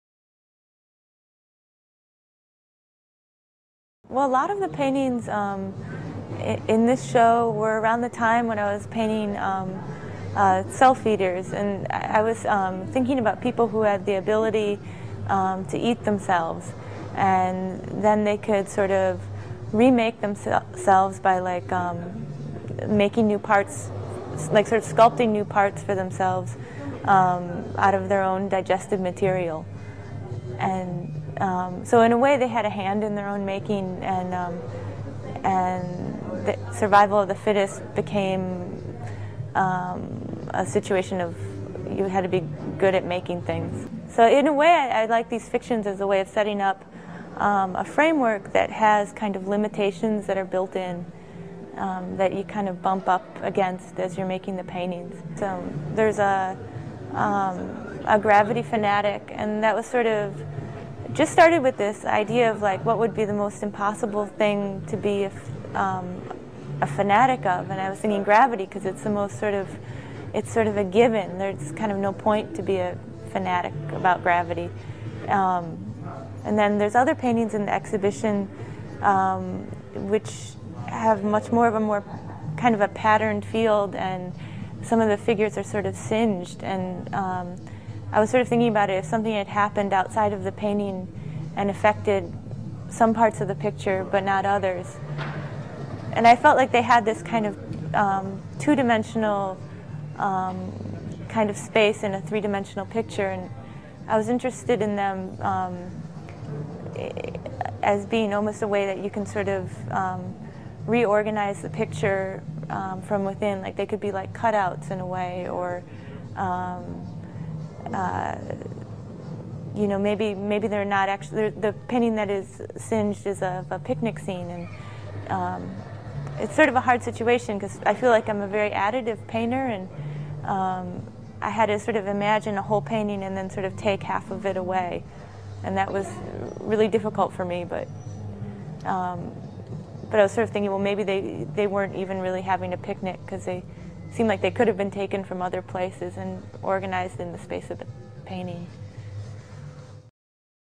Dana Schutz
In this short video excerpt Dana Schutz talks about some aspects of her art. To preview the Parkett publication featuring her work with multiple texts and numerous color illustrations as well as to view her specially made edition for Parkett please go to